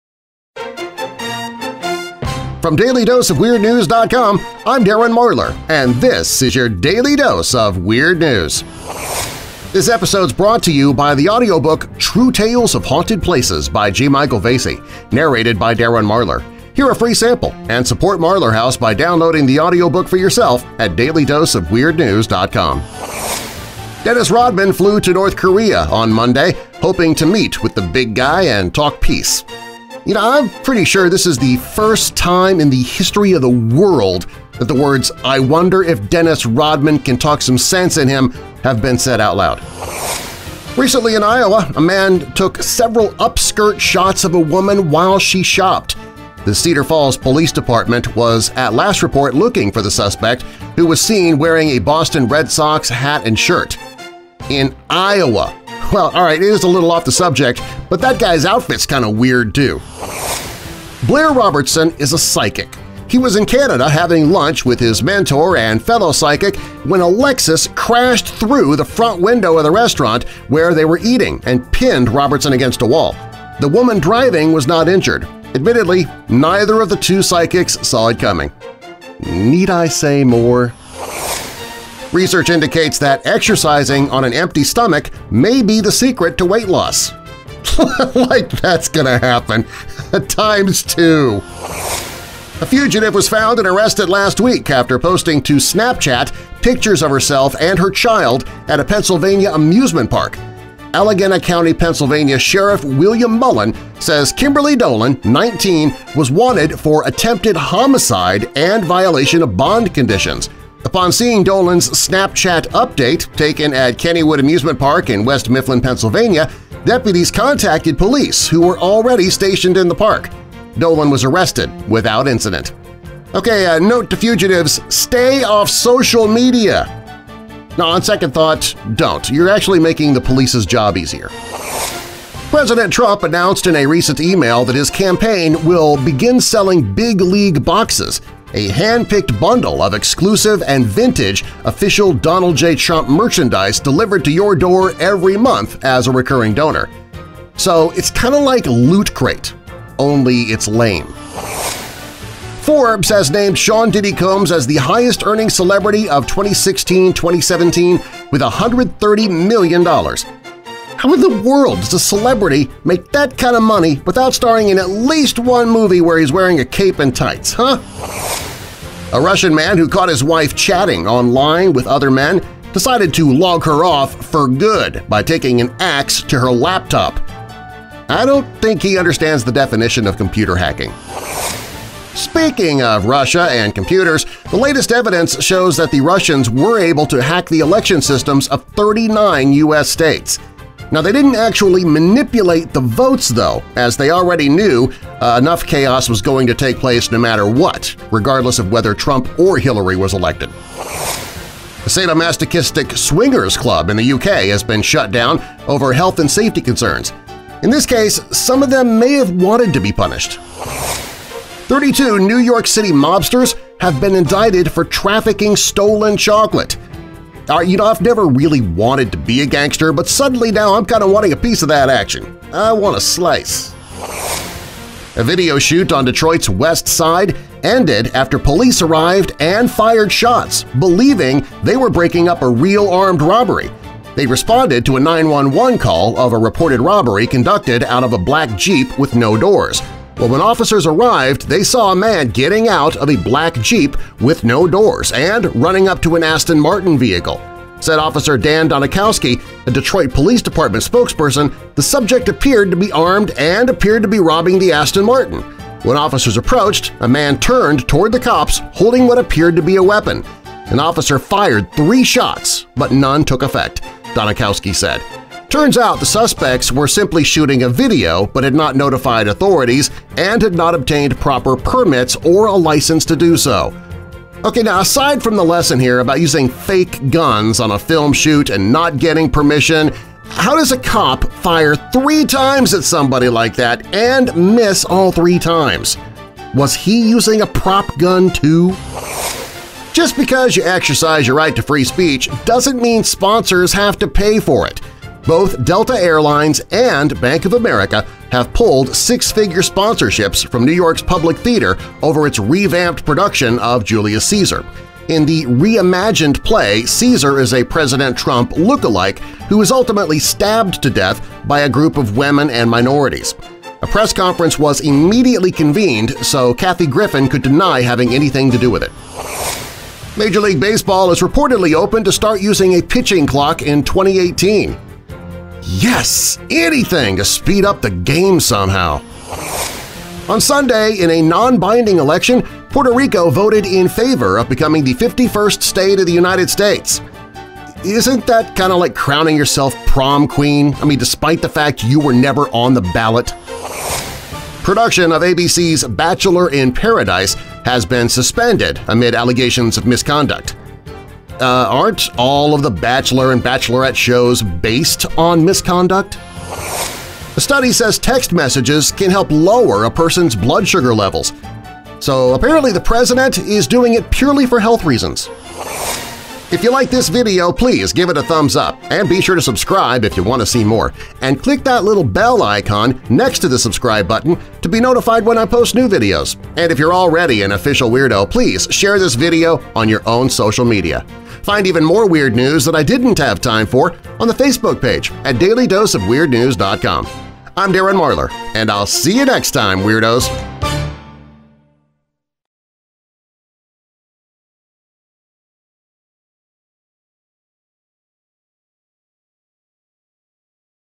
Psychics hit by car! * And 16 more true weird news stories!
Two psychics hit by car! * Snapchat captures wanted fugitive! * Will Puerto Rico become our 51st state? * The Mob indicted for stolen chocolate! * And MORE in this DAILY DOSE OF WEIRD NEWS!